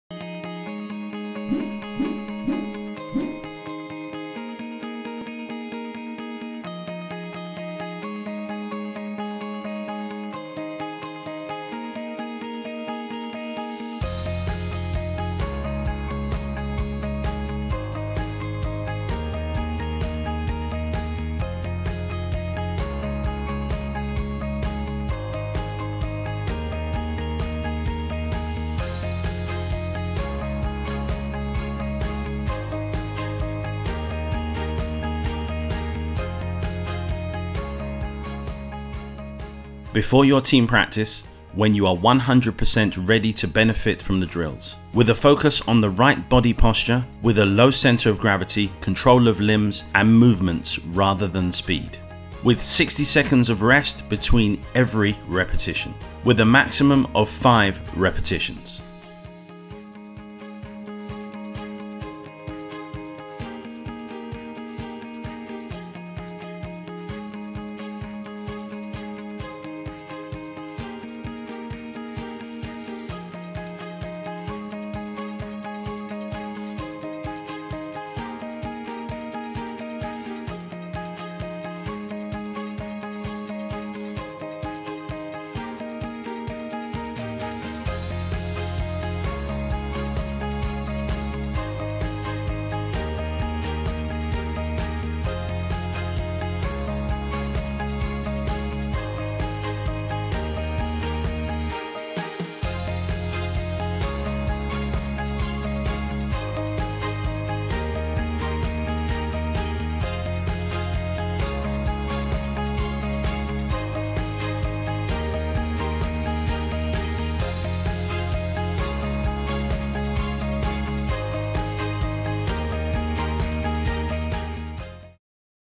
m-station Football Training | Physical Drills Balance | Sidestep and Jumping with low gravity
1. before your team practice when you are 100% ready to benefit from the drills.
2. with a focus on the right body posture with a low centre of gravity, control of limbs and movements rather than speed.
3. with 60 seconds of rest between every repetition.
4. with maximum 5 repetitions.